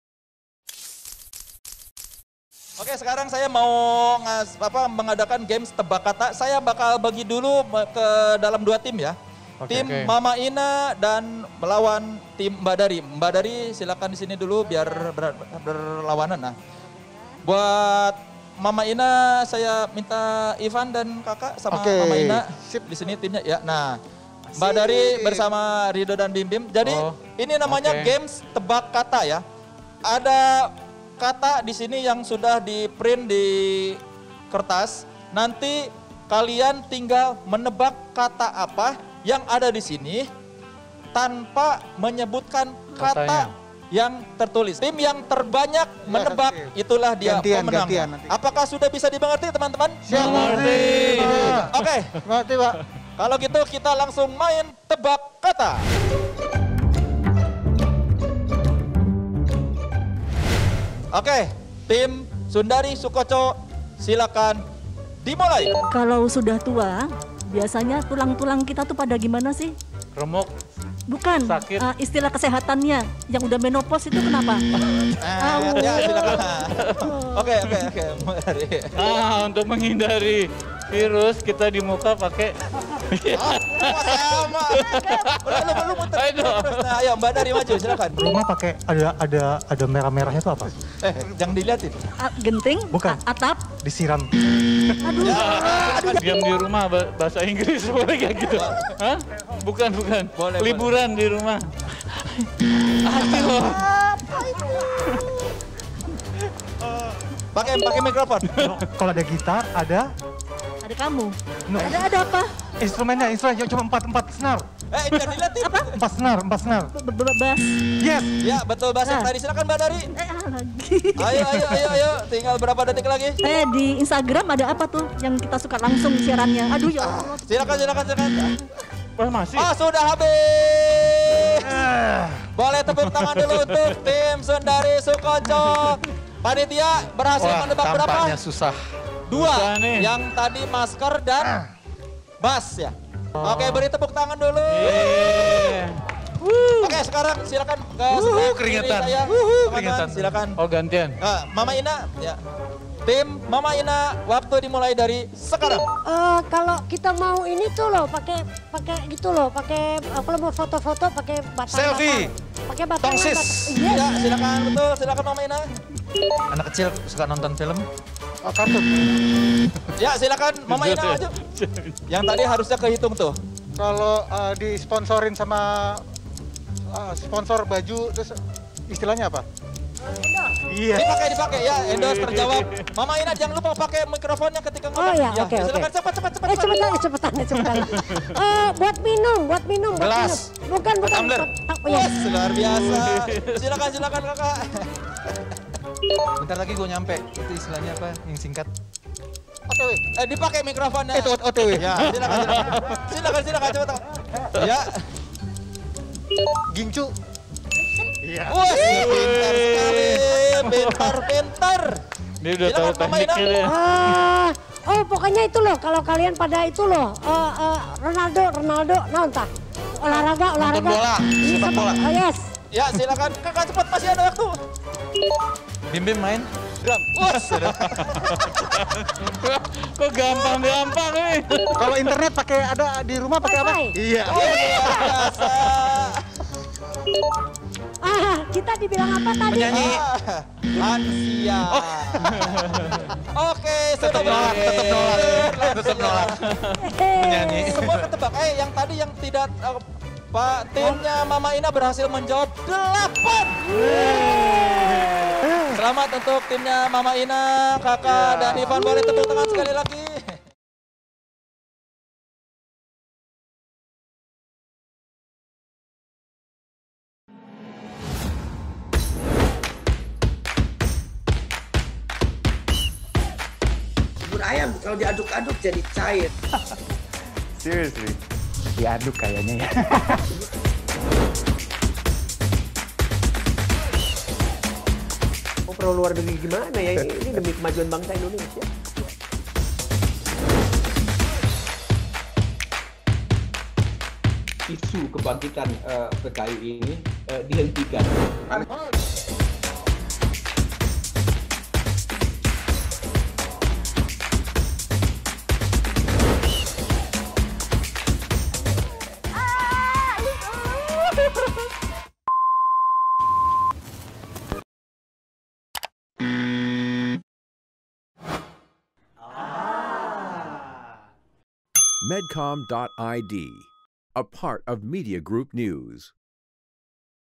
Vaksin Slank untuk Indonesia - Vina Panduwinata dan Sundari Soekotjo Diajak Main Tebak Kata
Personel Slank, Vina Panduwinata dan Sundari Soekotjo ditantang bermain tebak kata. Mereka dibagi menjadi dua kelompok dan harus bersaing menebak kata paling banyak. Tim A terdiri dari Sundari Soekotjo, Bimbim dan Ridho. Tim B terdiri dari Vina Panduwinata, Kaka dan Ivan. Mau tau keseruannya? Yuk, simak videonya!